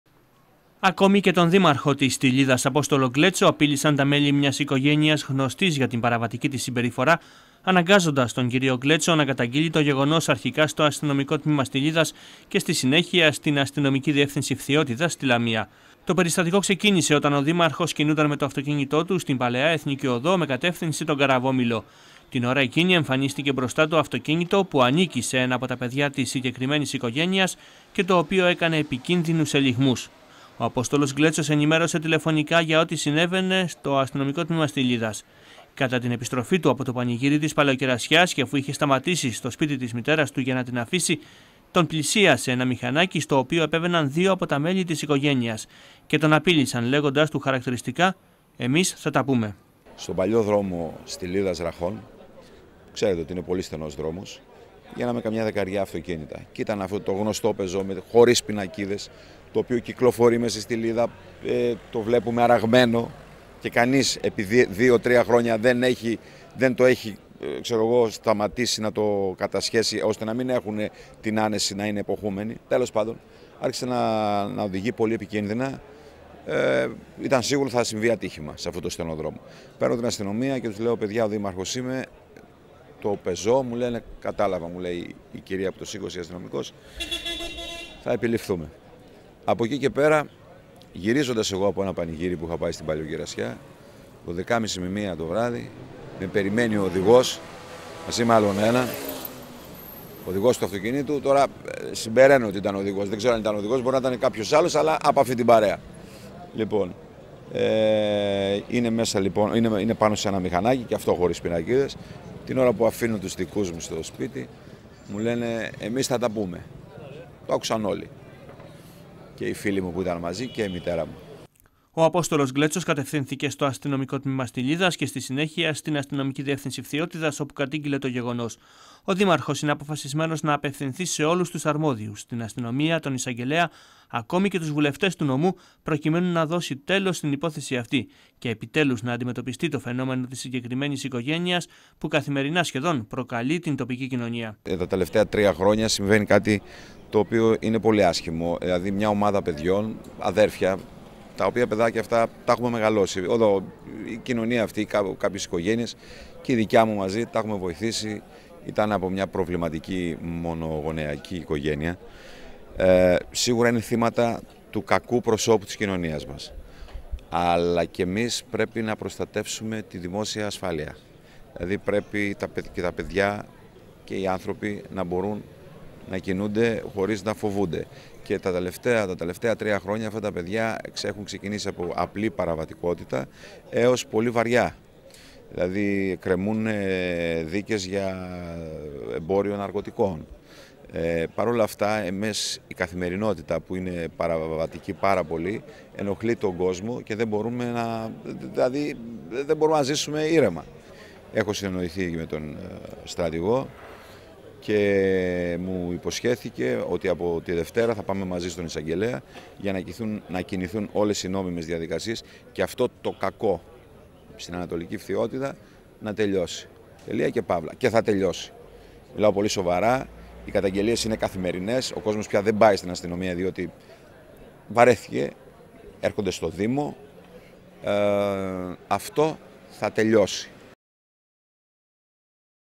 Στον εισαγγελέα ο Απόστολος Γκλέτσος mp4
Απειλές από νεαρούς κατοίκους της Στυλίδας καταγγέλει ότι δέχθηκε ο ο Απόστολος Γκλέτσος. Οι δράστες, που απασχολούν καθημερινά την τοπική κοινωνία με την παράνομες πράξεις τους συνάντησαν τα ξημερώματα του Σαββάτου τον δήμαρχο και τον απείλησαν.
Μιλώντας στο STAR ο κύριος Γκλέτσος δηλώνει πως η υπόθεση με τα συγκεκριμένα παιδιά, που μάλιστα είναι αδέρφια θα τελείωσει σύντομα. Αύριο μάλιστα θα ζητήσει την συνδρομή της δικαιοσύνης αφού θα βρεθεί στο γραφείο του εισαγγελεά Λαμίας.